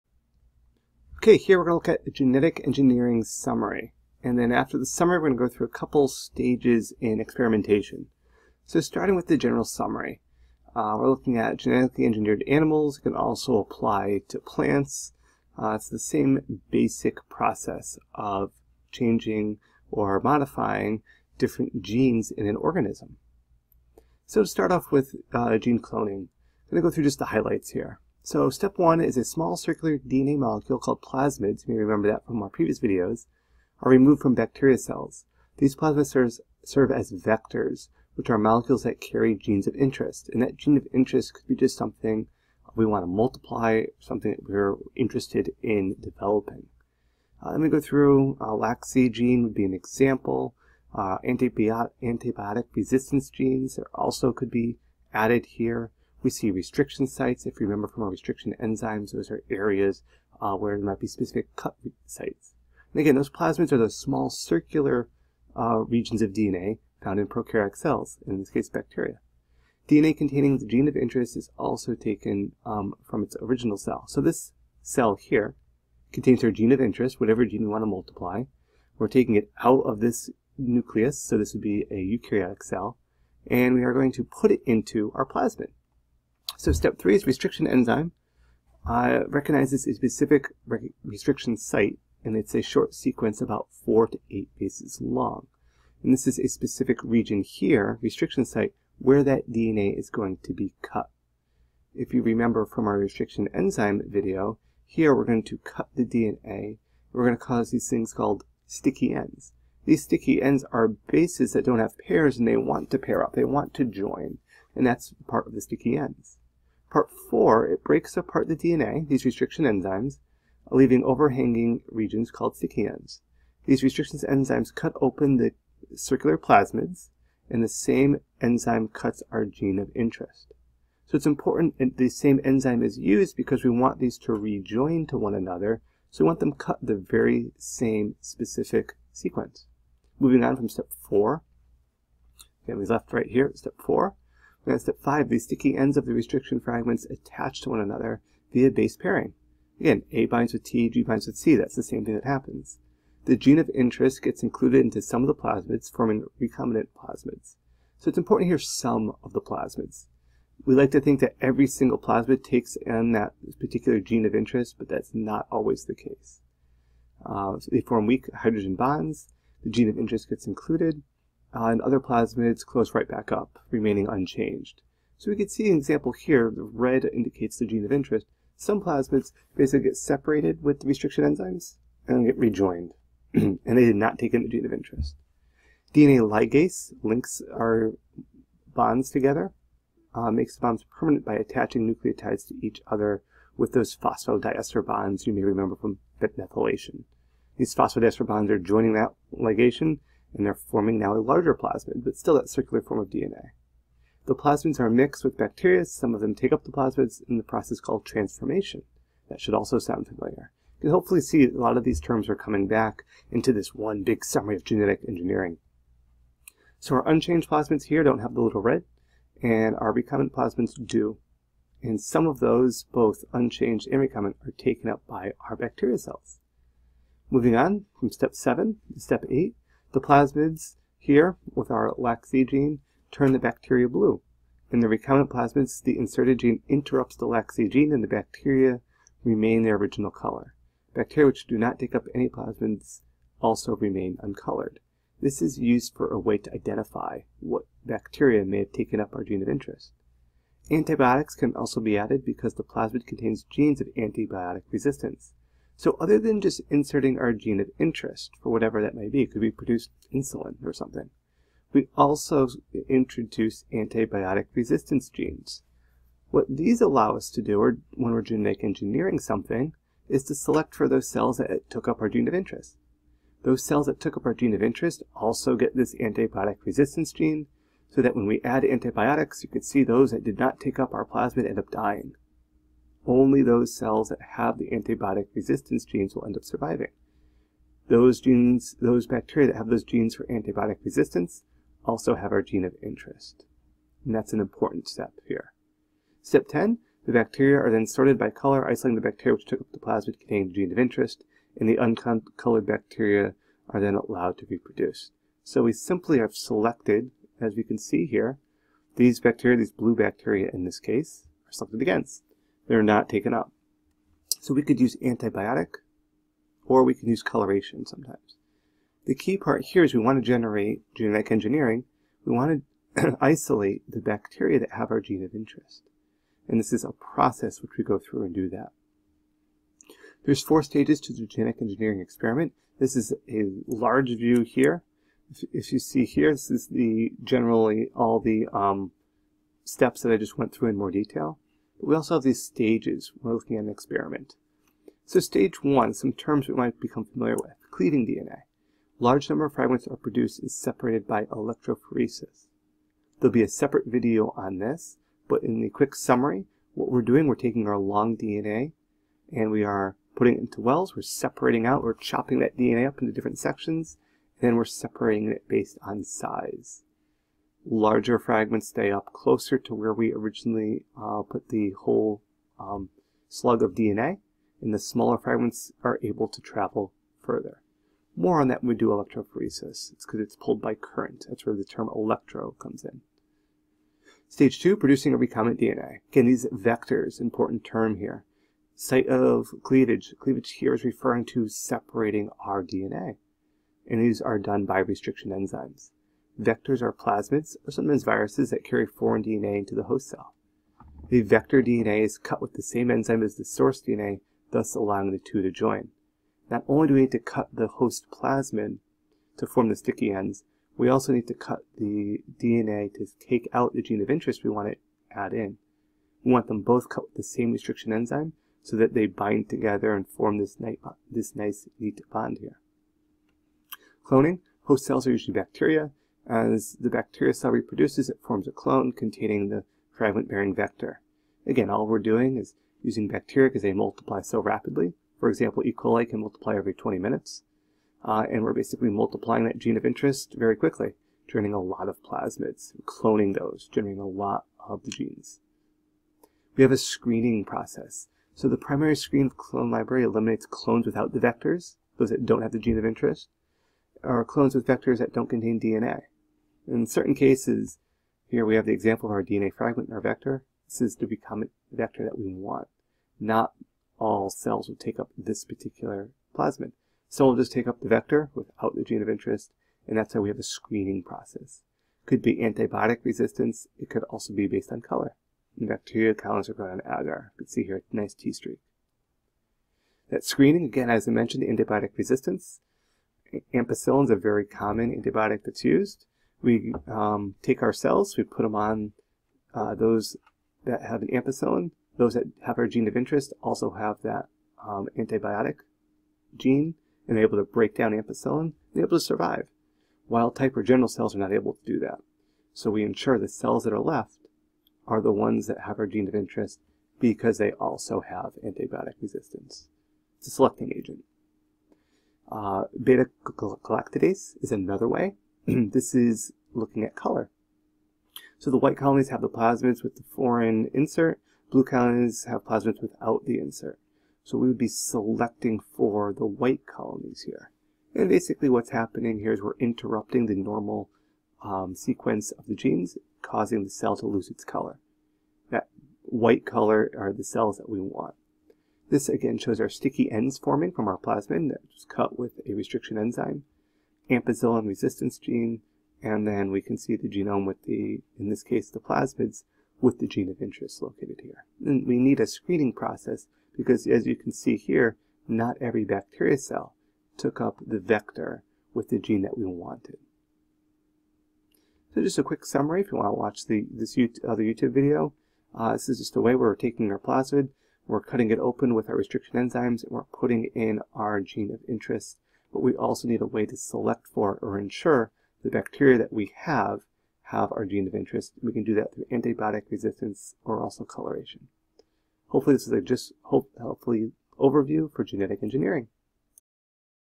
Genetic Engineering Summary
Fragments of source DNA are inserted into vectors typically plasmids because the host cells are usually bacteria.  This is accomplished via restriction enzymes to generate sticky ends.

A preliminary screen of the clone library eliminates
-Clones without vectors
-Clones with vectors that do not contain DNA

Antibiotic resistance commonly to ampicillin can be used as a selecting agent, but b-galactosidase can also be used.  The proteins can be used in research, heat attack therapies,  human growth hormones.

*Due to the description character limit the full work cited for "Genetic Engineering Summary" can be viewed